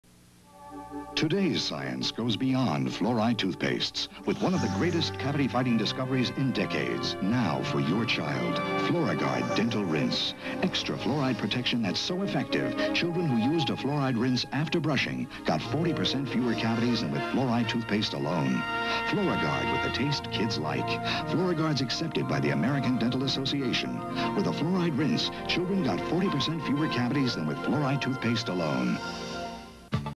Colgate Flourigard ad, 1983
Outer space-themed spot for the brand's dental rinse.